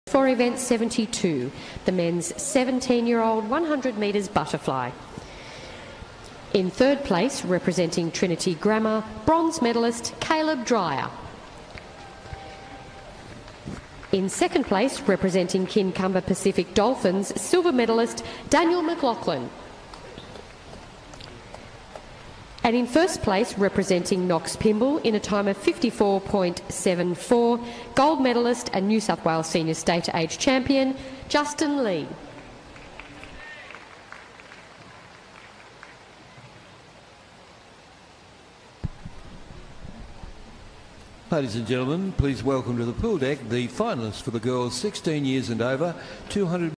2022 State Age Championships - Boys 17 100m Butterfly Final Justin Lee 🥇 Ceremony
Justin Lee